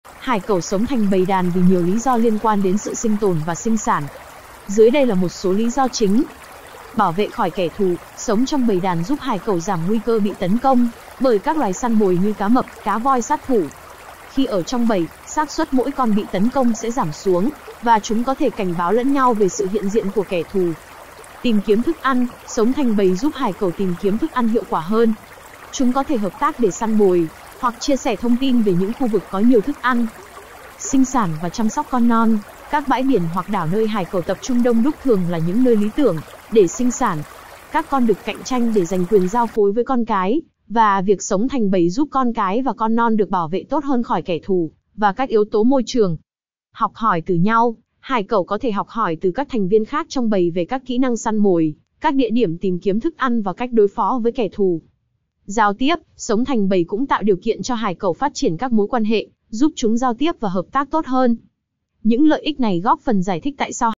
Vì Sao Hải Cẩu Lại Tập Trung Thành Bầy Đàn
Hải cẩu sống thành bầy đàn vì nhiều lý do liên quan đến sự sinh tồn và sinh sản. Dưới đây là một số lý do chính:

Bảo vệ khỏi kẻ thù: Sống trong bầy đàn giúp hải cẩu giảm nguy cơ bị tấn công bởi các loài săn mồi như cá mập, cá voi sát thủ. Khi ở trong bầy, xác suất mỗi con bị tấn công sẽ giảm xuống, và chúng có thể cảnh báo lẫn nhau về sự hiện diện của kẻ thù.

Tìm kiếm thức ăn: Sống thành bầy giúp hải cẩu tìm kiếm thức ăn hiệu quả hơn. Chúng có thể hợp tác để săn mồi hoặc chia sẻ thông tin về những khu vực có nhiều thức ăn.

Sinh sản và chăm sóc con non: Các bãi biển hoặc đảo nơi hải cẩu tập trung đông đúc thường là những nơi lý tưởng để sinh sản. Các con đực cạnh tranh để giành quyền giao phối với con cái, và việc sống thành bầy giúp con cái và con non được bảo vệ tốt hơn khỏi kẻ thù và các yếu tố môi trường.

Học hỏi từ nhau: Hải cẩu có thể học hỏi từ các thành viên khác trong bầy về các kỹ năng săn mồi, các địa điểm tìm kiếm thức ăn và cách đối phó với kẻ thù.

Giao tiếp xã hội: Sống thành bầy cũng tạo điều kiện cho hải cẩu phát triển các mối quan hệ xã hội, giúp chúng giao tiếp và hợp tác tốt hơn.

Những lợi ích này góp phần giải thích tại sao hải cẩu thường sống thành bầy đàn.
kênh dạy excel từ cơ bản đến nâng cao đầu tiên tại Việt Nam.